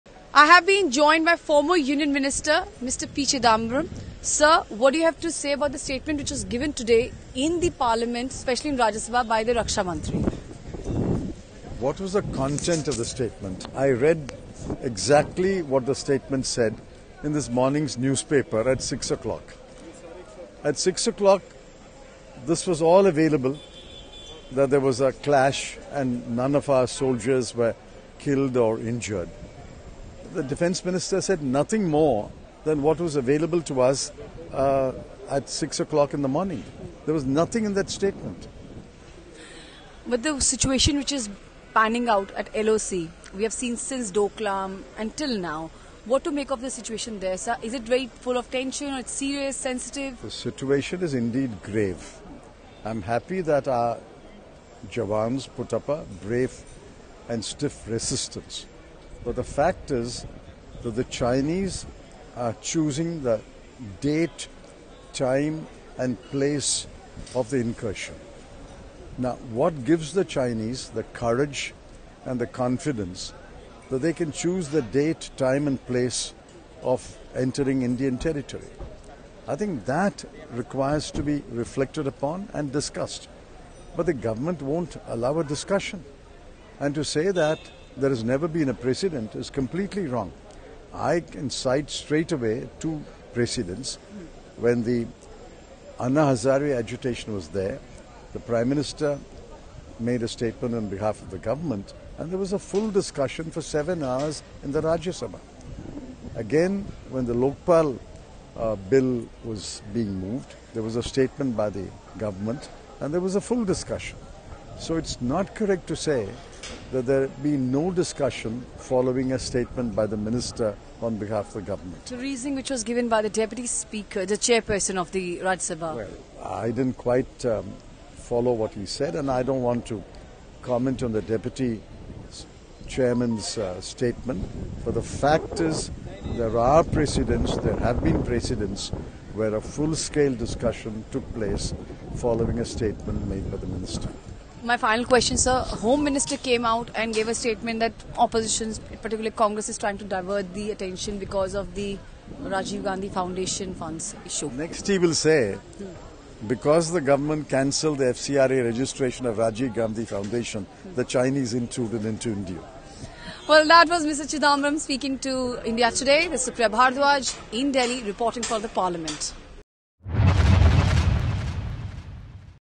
Chidambaram Downplays Rajnath Singh's Statement on India China Clash, Cites Manmohan Singh's Example
P Chidambaram proactively demanded a discussion on the border issue amid India china faceoff .